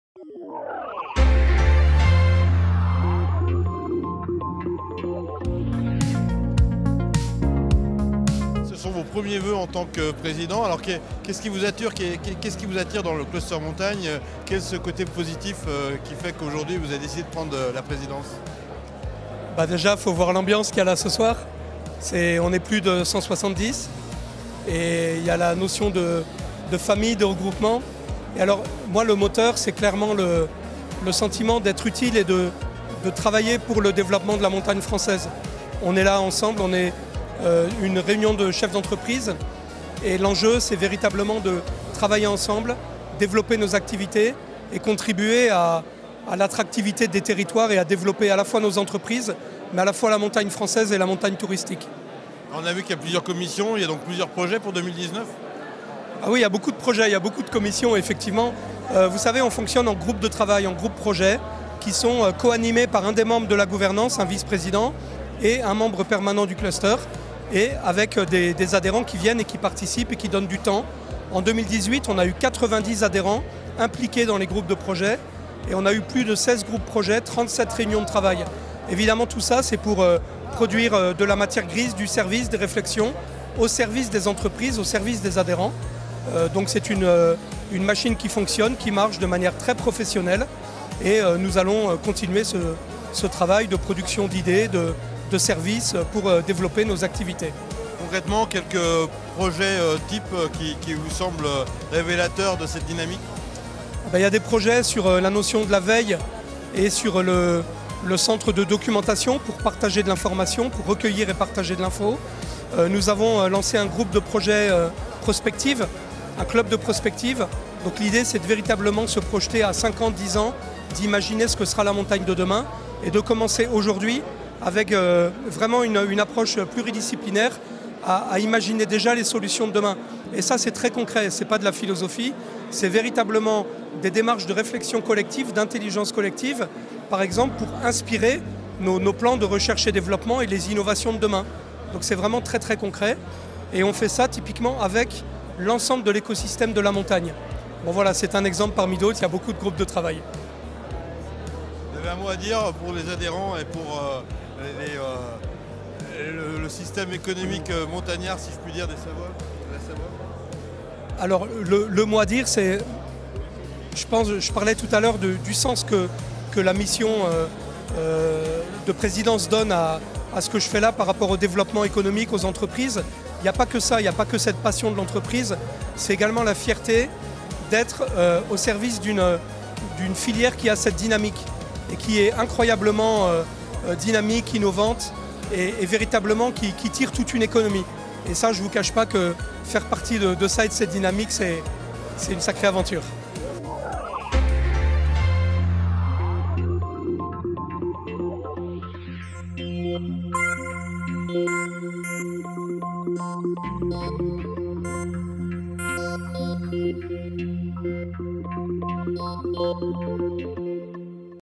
Montmélian : Cluster Montagne : les projets en 2019
Interview du Président sur les actions et projets du Cluster Montagne.
Un reportage Defours.com